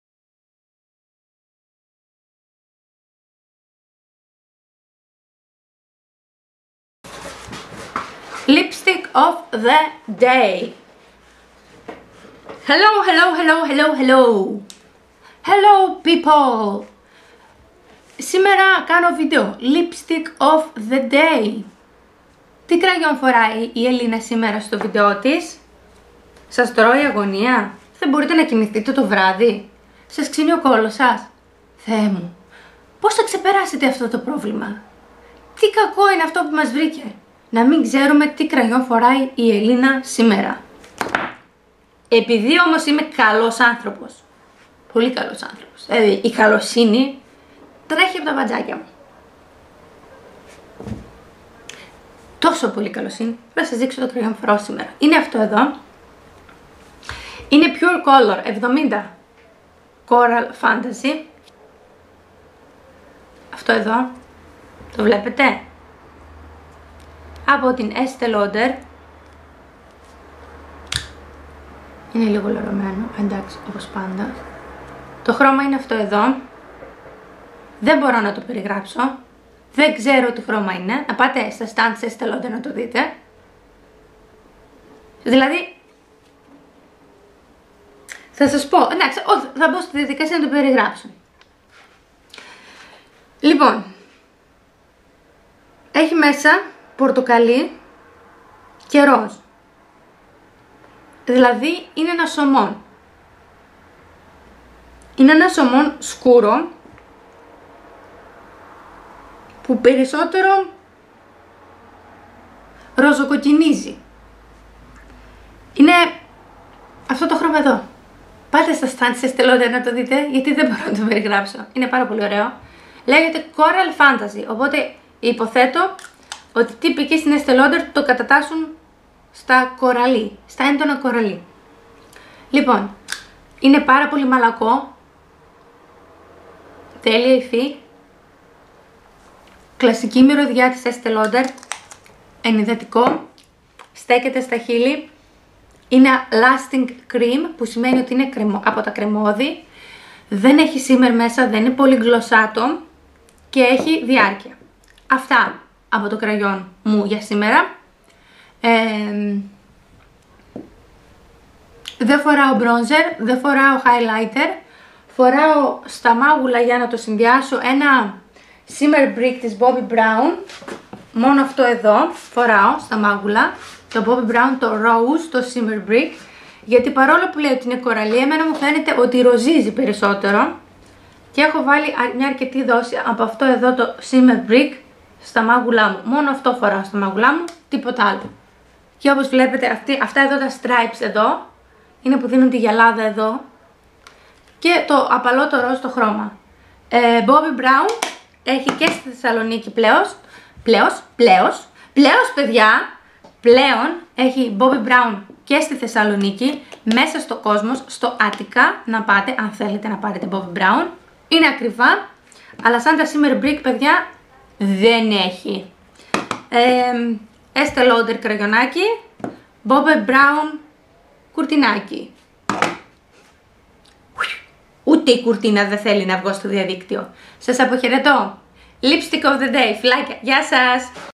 Κραγιον και ρουζ -03.10.2012
Επιλογες μου για το μακιγιαζ της ημερας!